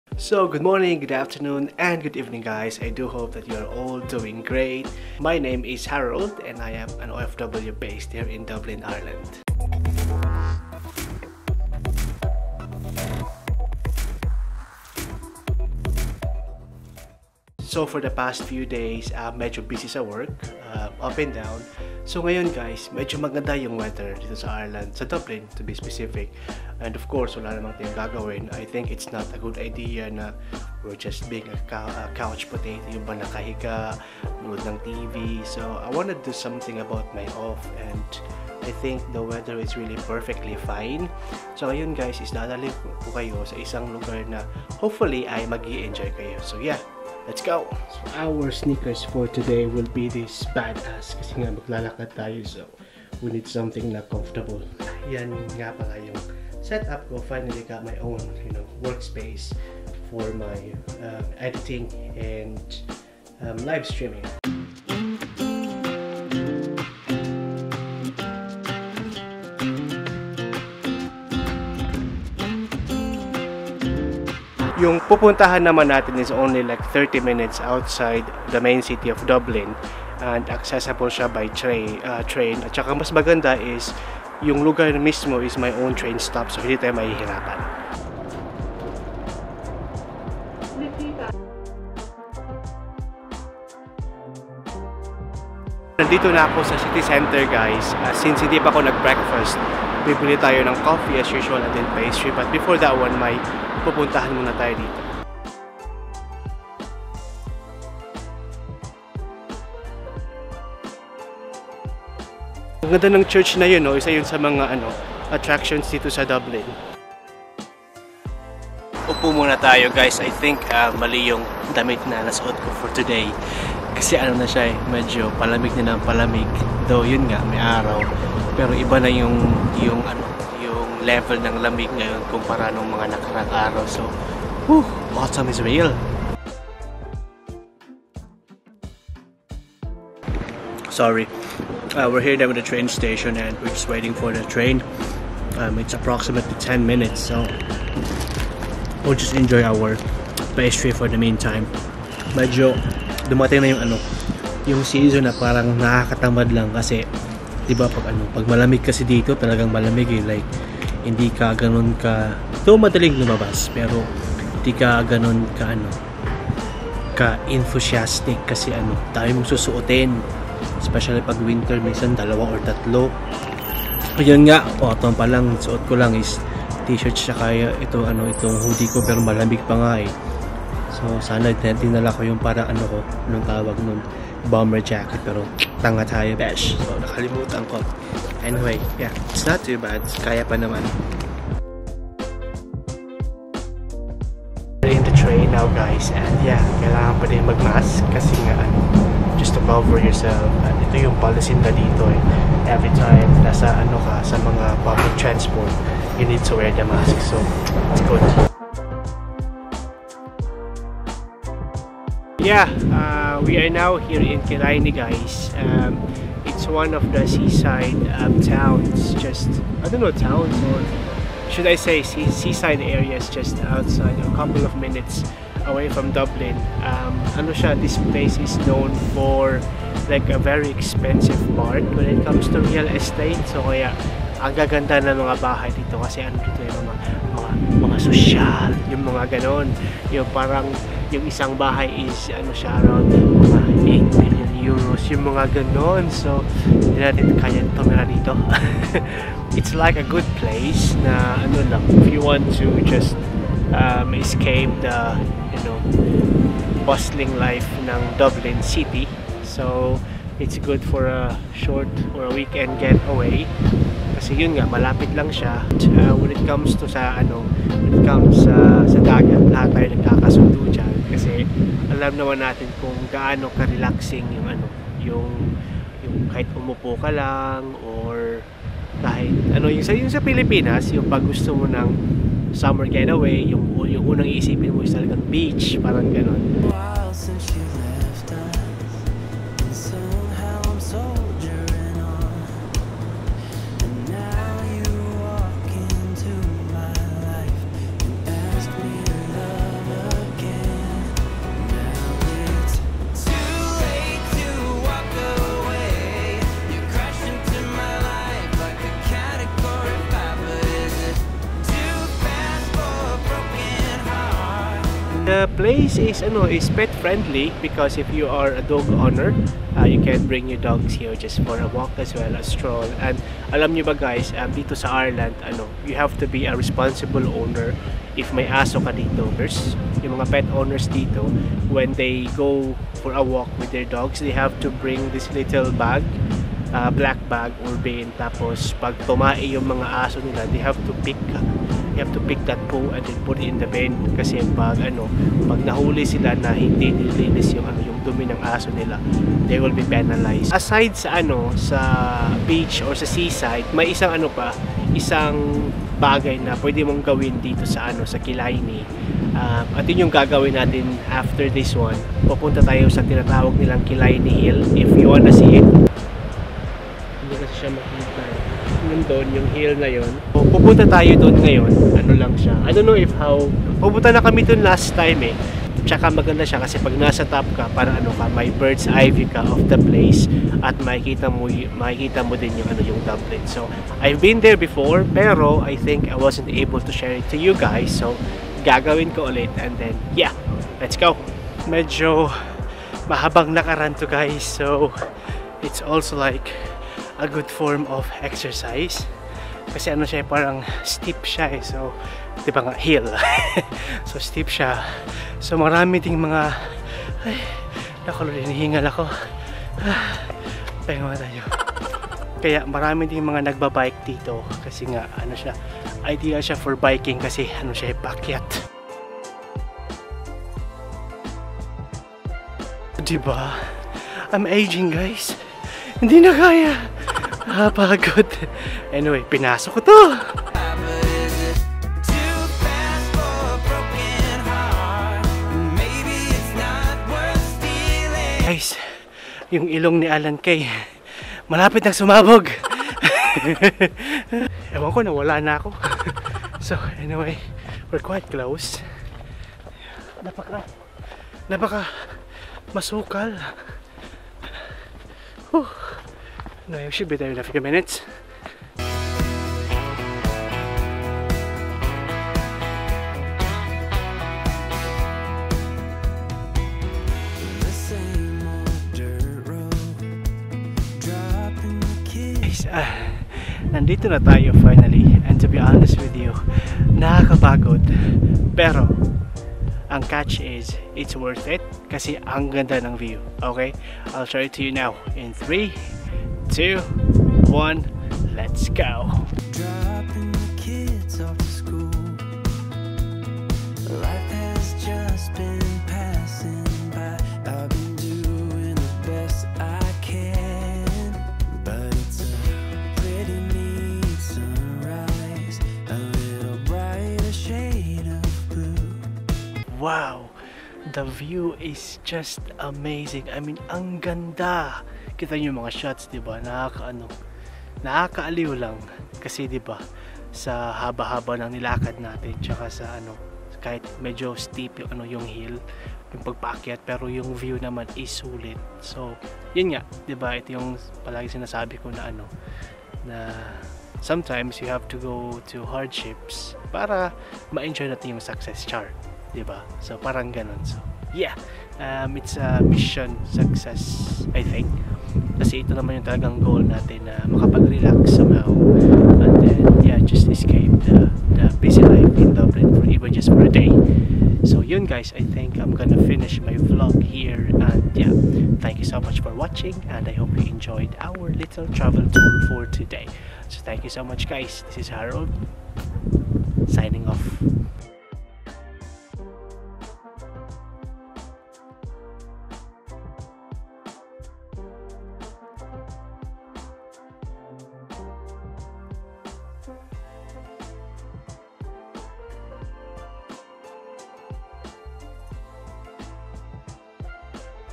VLOG 33 - KILLINEY HILL| KILLINEY BEACH | COUNTY DUBLIN | TRAVEL VLOG
In this video guys, I am happy to take you for a stroll on the beautiful place of Killiney! Hopefully you will enjoy it!

I would also like to thank Amazing Janus for the beautiful thumbnail check his link below guys.